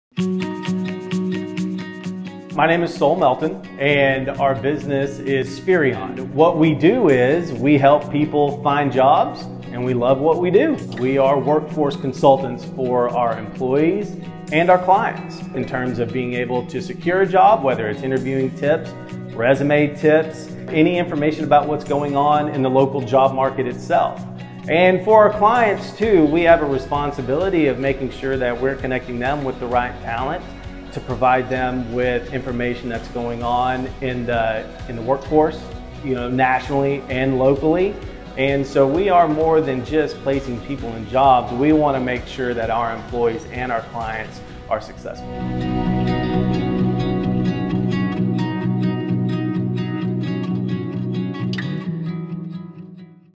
These guys know how to find great jobs for locals!
What does Spherion do? They find jobs for people and people for jobs! They have been loved in the Waco and Temple Belton area for 8 years, so they are a well trusted source if you are looking for a job change!